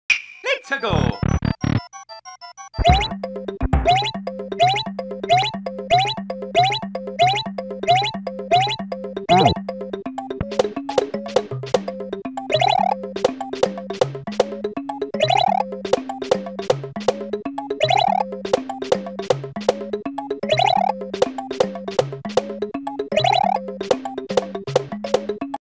Silly P-Consistency Preview
I have no idea if I will upload or even finish this level, but I really enjoyed p-switch maps recently. So I thought I might do my own with adding some pressure for the jumps :D